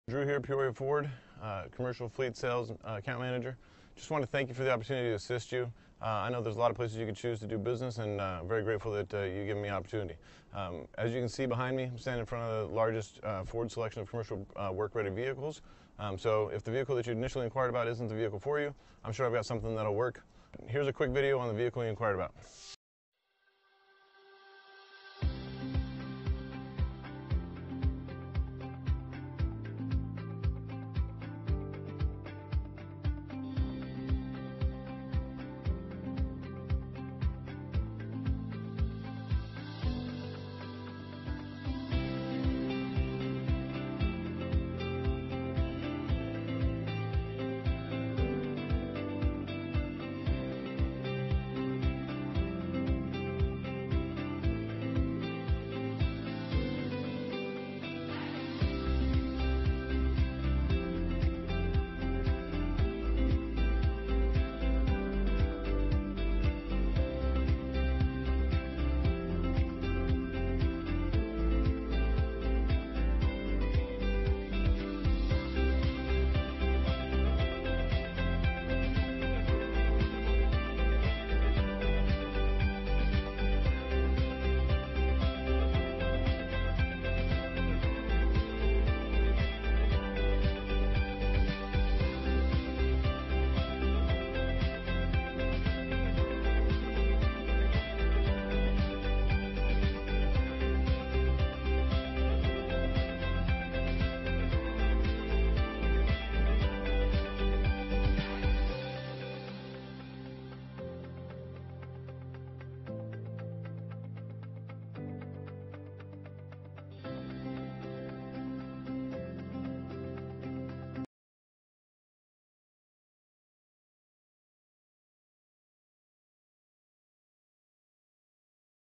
2022 Ford Super_Duty_F-250_SRW NEF64876
Oxford White New 2022 Ford Super_Duty_F-250_SRW  available in Peoria, Arizona at Peoria Ford Fleet.

9130 W. Bell Rd

4WD. Oxford White 2022 Ford F-250SD XLT Fleet 10-Speed Automatic Power Stroke 6.7L V8 DI 32V OHV Turbodiesel 4WDThank you for looking. At Peoria Ford we are here to serve.